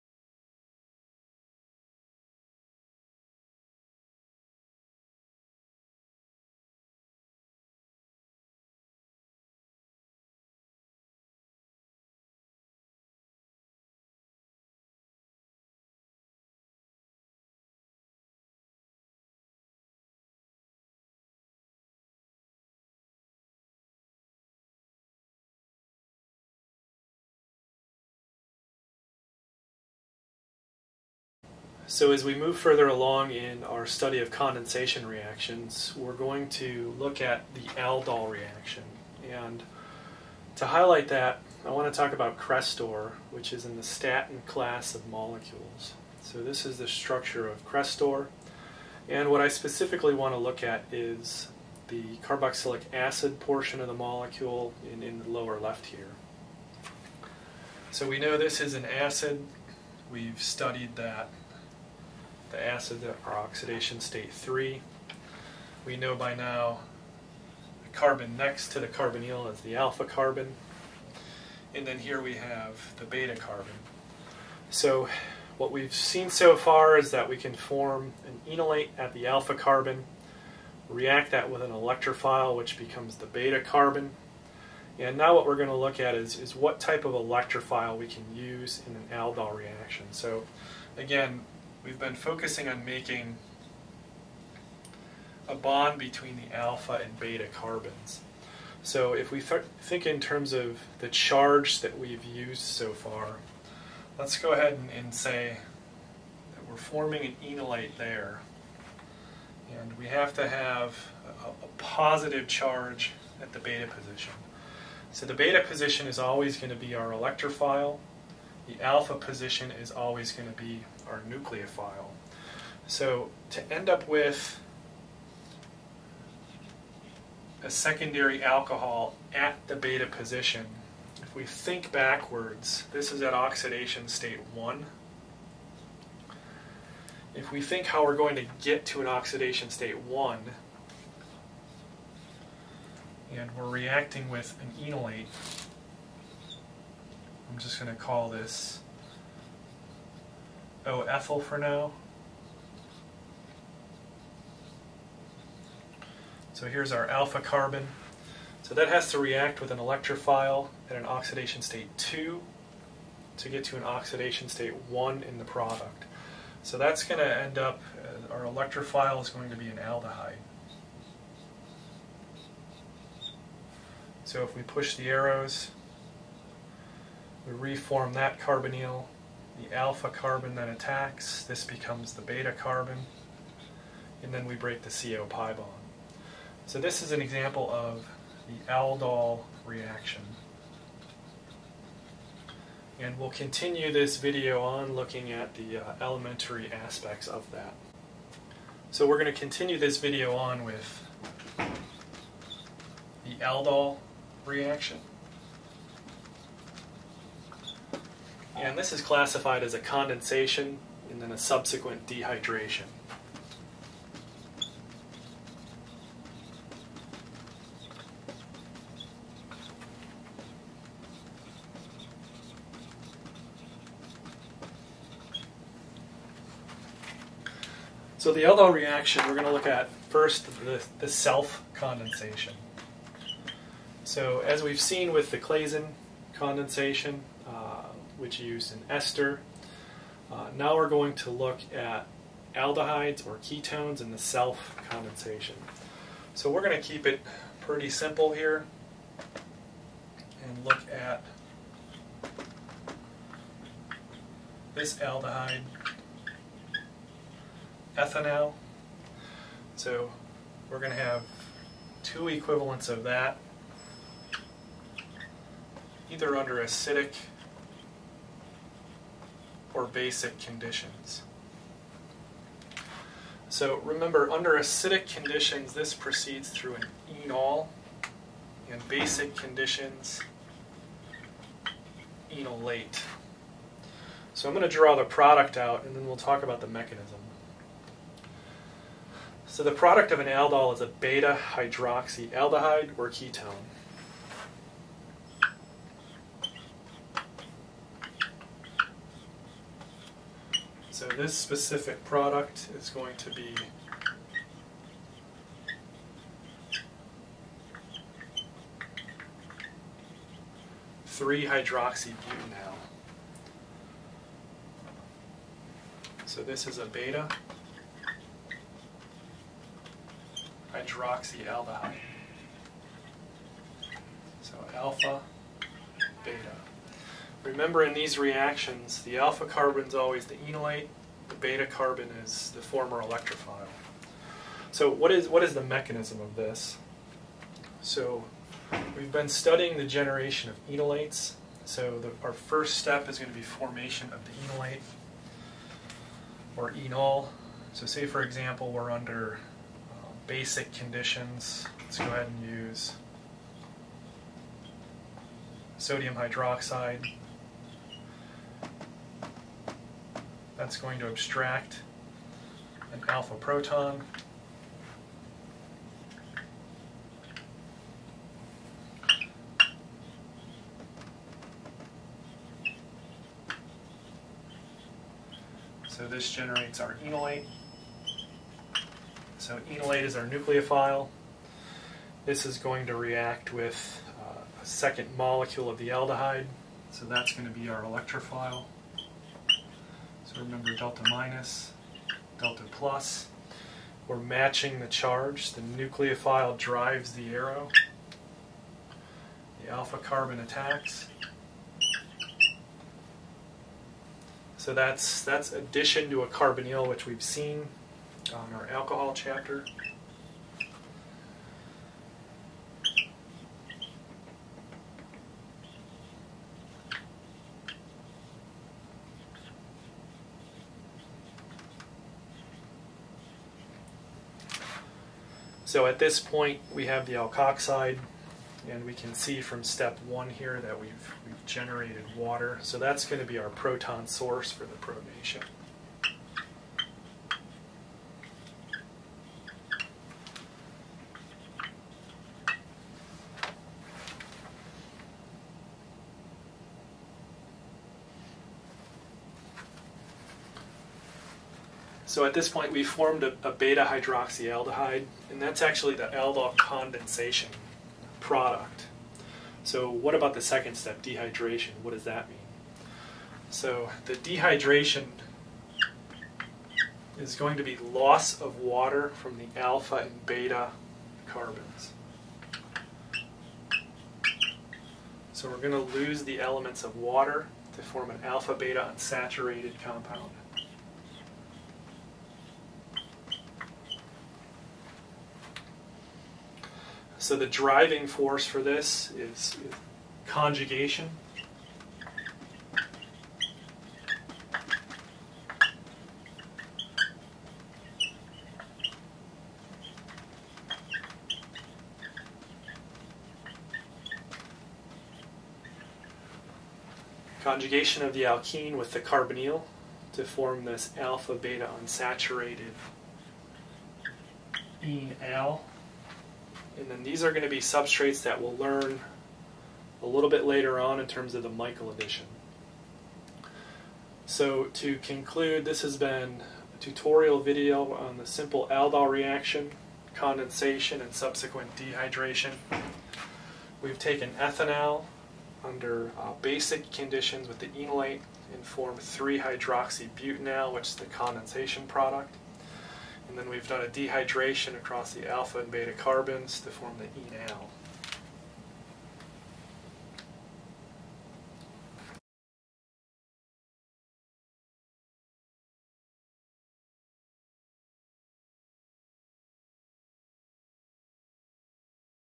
Aldol Condensation-Dehydration in Organic Chemistry
This tutorial video examines the self aldol condensation of an aldehyde to produce a beta-hydroxyaldehyde. The mechanism is discussed in basic media through formation of an enolate that reacts with an aldehyde. The enolate is the nucleophile and the aldehyde is the electrophile so the new bond that is formed is now the alpha-beta C-C bond. Aldol condensation products can dehydrate by loss of the elements of water across the alpha and beta carbons to give a conjugated aldehyde, called an enal. The reaction holds true for ketones as well.